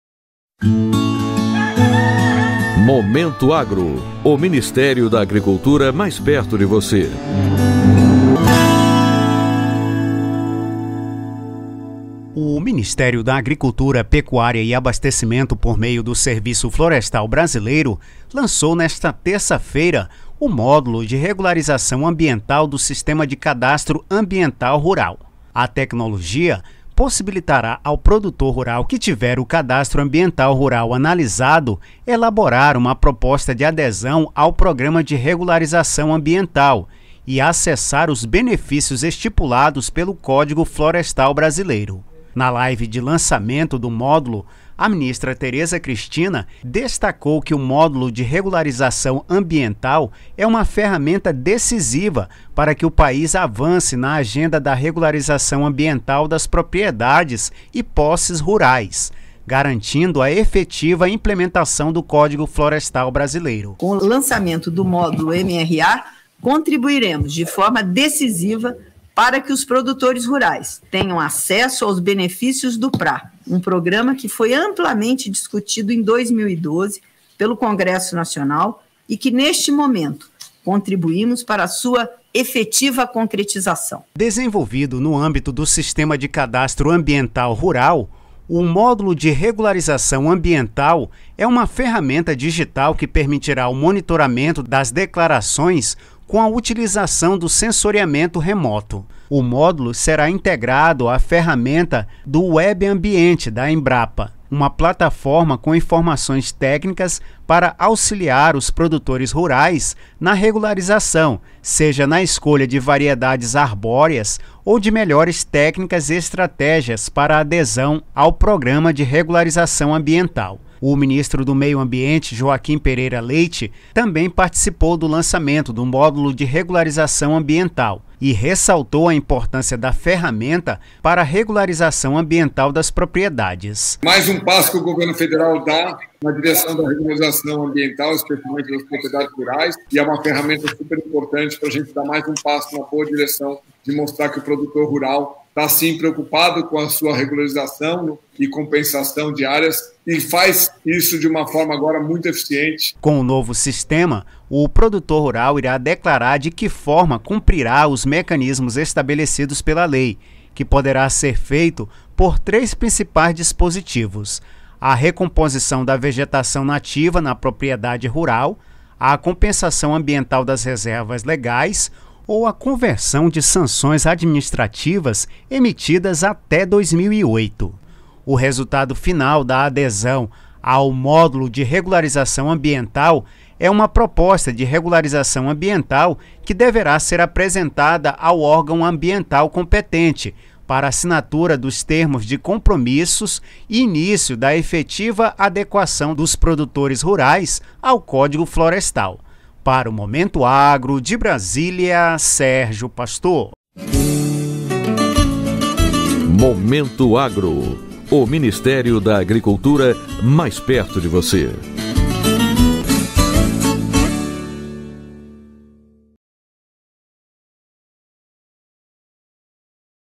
21/12/2021 MINISTÉRIO AGRICULTURA LANÇA FERRAMENTA P/ FACILITAR REGULARIZAÇÃO AMBIENTAL|Momento Agro
O Ministério da Agricultura, Pecuária e Abastecimento (Mapa), por meio do Serviço Florestal Brasileiro, lançou nesta terça-feira (21) o Módulo de Regularização Ambiental do Sistema de Cadastro Ambiental Rural (Sicar). A tecnologia possibilitará ao produtor rural que tiver o Cadastro Ambiental Rural (CAR) analisado elaborar uma proposta de adesão ao Programa de Regularização Ambiental (PRA) e acessar os benefícios estipulados pelo Código Florestal Brasileiro.

Na live de lançamento do Módulo, a ministra Tereza Cristina destacou que o MRA é uma ferramenta decisiva para que o país avance na agenda da regularização ambiental das propriedades e posses rurais, garantindo a efetiva implementação do Código Florestal Brasileiro.

Desenvolvido no âmbito do Sicar, o MRA é uma ferramenta digital que permitirá o monitoramento das declarações com a utilização de sensoriamento remoto. O MRA será integrado à ferramenta do WebAmbiente da Embrapa, uma plataforma com informações técnicas para auxiliar os produtores rurais na regularização, seja na escolha de variedades arbóreas, ou de melhores técnicas e estratégias para sua adesão ao PRA.

O Ministro do Meio Ambiente, Joaquim Pereira Leite, também participou do lançamento do MRA, e ressaltou a importância da ferramenta para regularização ambiental das propriedades.

Com o novo sistema, o produtor rural irá declarar de que forma cumprirá os mecanismos estabelecidos pela lei, que poderá ser feito por três principais dispositivos: a recomposição da vegetação nativa na propriedade rural, a compensação ambiental das reservas legais ou a conversão de sanções administrativas emitidas até 2008. O resultado final da adesão ao MRA é uma proposta de regularização ambiental que deverá ser apresentada ao órgão ambiental competente para a assinatura dos Termos de Compromissos e início da efetiva adequação dos produtores rurais ao Código Florestal.

registrado em